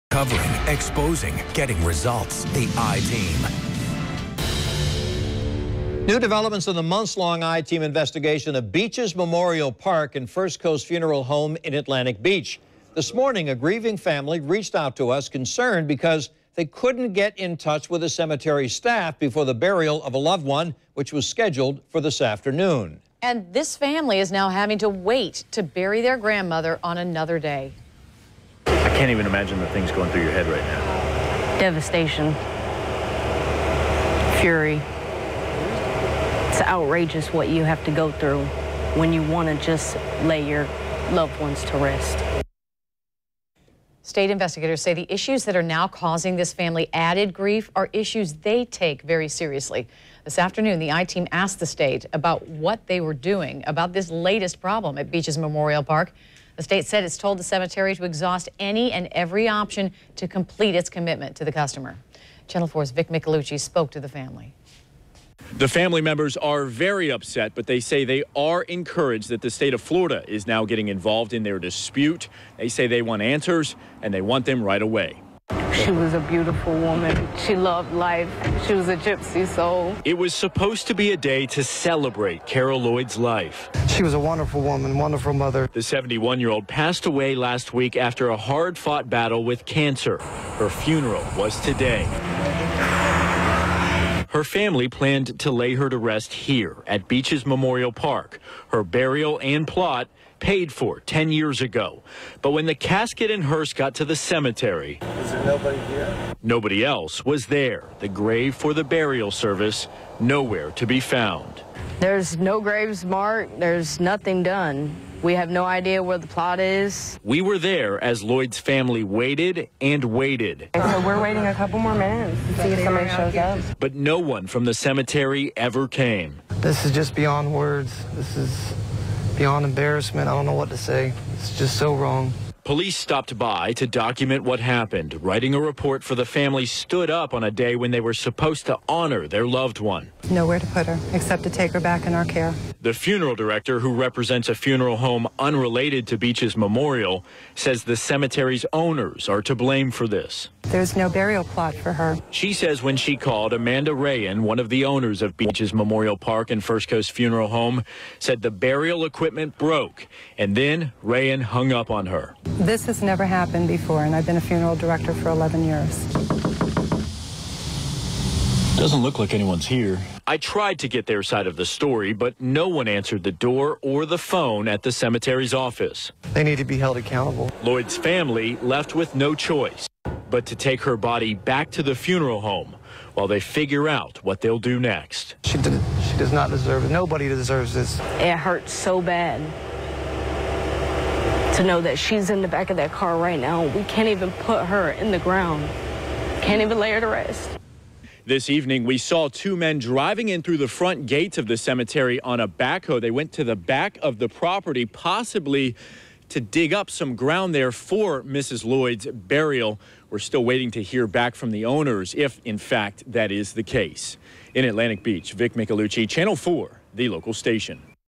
I-TEAM: Family unable to bury loved one
VIDEO: New developments unfold in the I-TEAM investigation into the Beaches Memorial Park and First Coast Funeral Home in Atlantic Beach. A grieving family reached out to the I-TEAM, concerned because they couldn't get in touch with the cemetery staff before their loved one's burial.